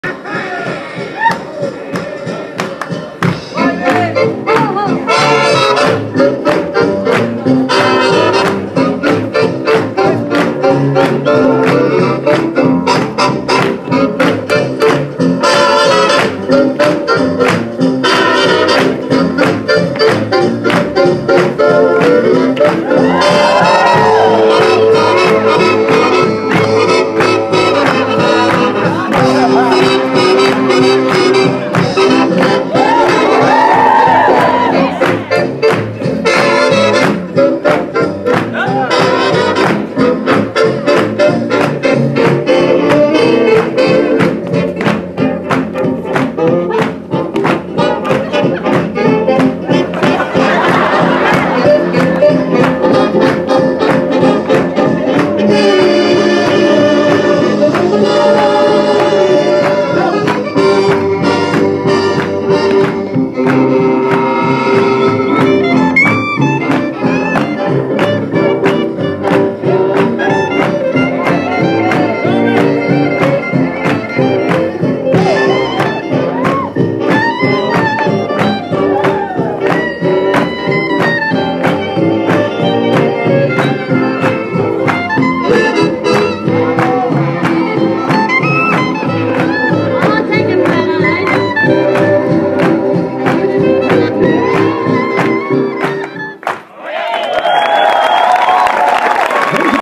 The Snowball 2013 - Invitational Strictly - Vincenzo & Katja
Global Swing Broadcast - Snowball 2013 Video Broadcast Series
The Snowball in Stockholm, Dec 26 2013 - Jan 2 2014

Invitational Strictly Competition
1st Place - Skye & Frida
2nd Place - Peter & Naomi
3rd Place - Nikolas & Mikaela
4th Place - Nick & Alice
5th Place - Henric & Joanna Stillman
6th Place - Remy & Jessica
7th Place - Vincenzo & Katja
8th Place - Andreas & Mette